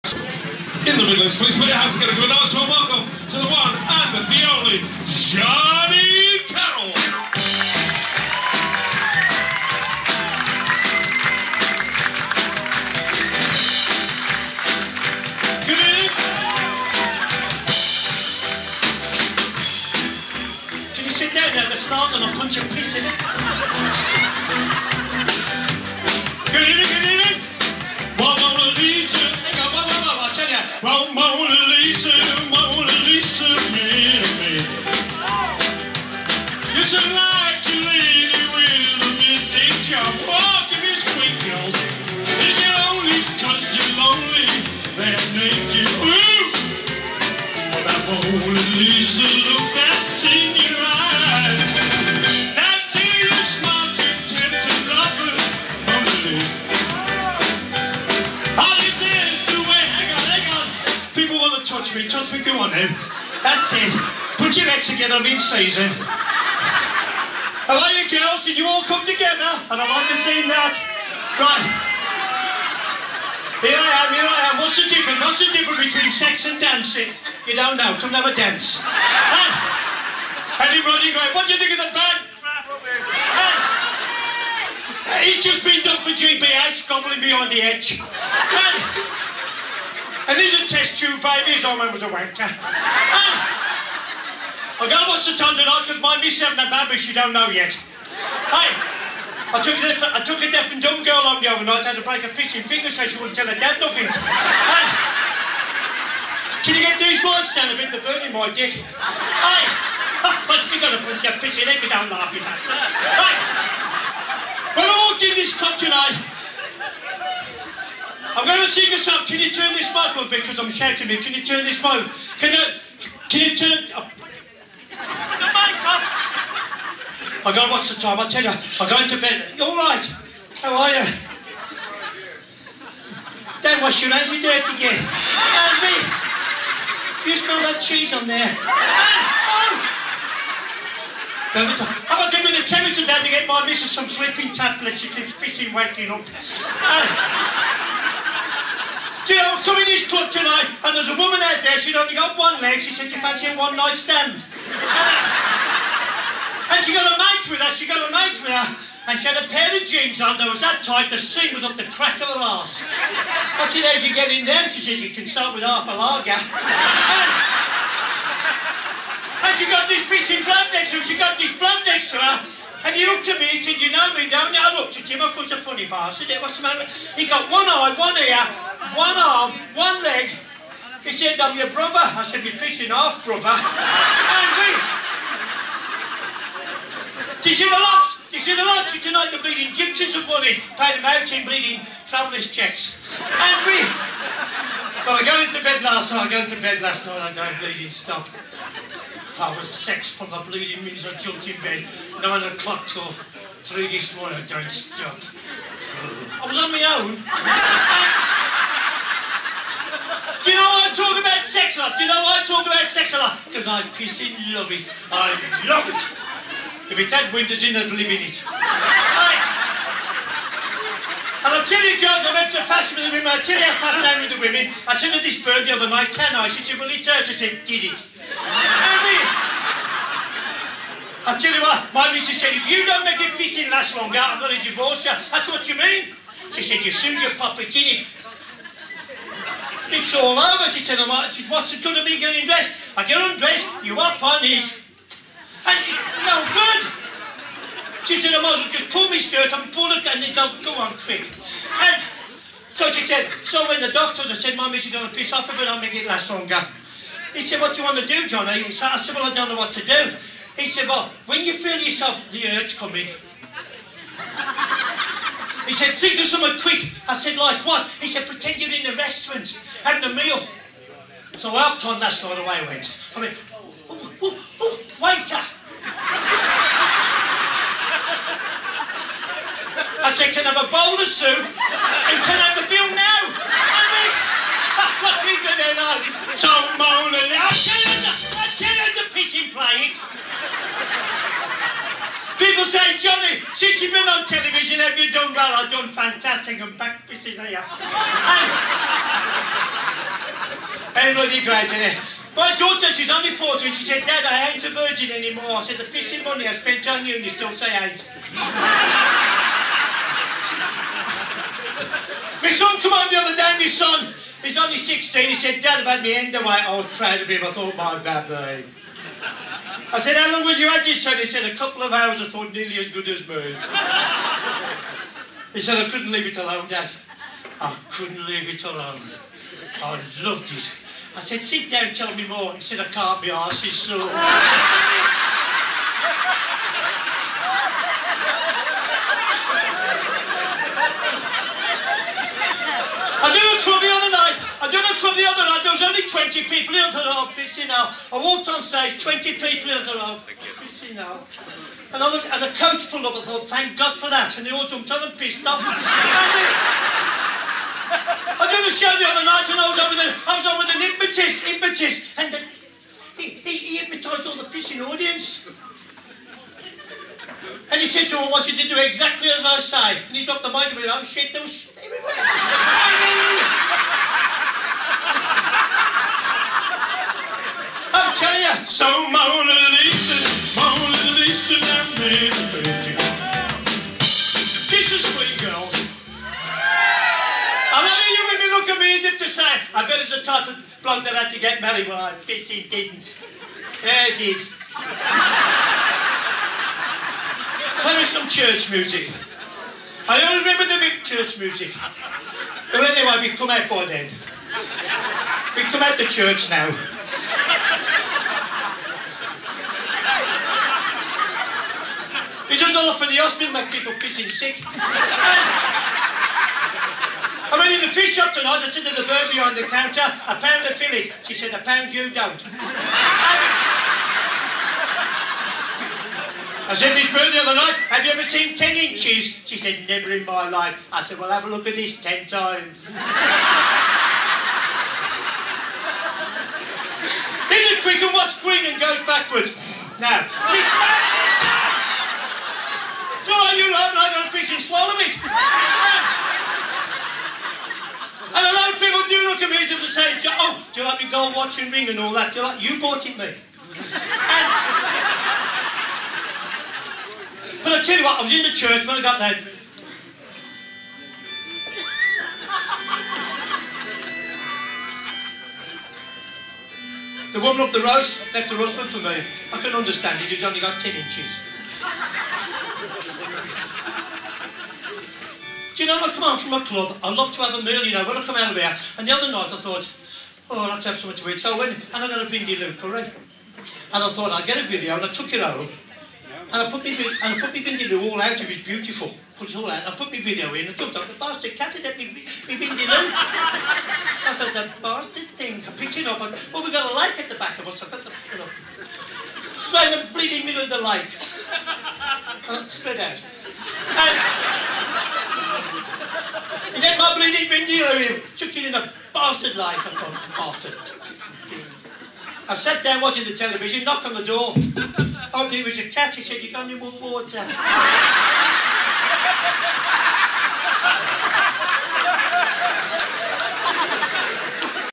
"JOHNNY CARROLL" Live at THE BARN BIRMINGHAM 2008 (extreme adult humour and non-PC -over 18's only)
The brilliant and charismatic Birmingham Comedian "Johnny Carroll" always performed to 'packed out' venues and clubs around the UK. He sadly passed away January 2020. Gone but not forgotten.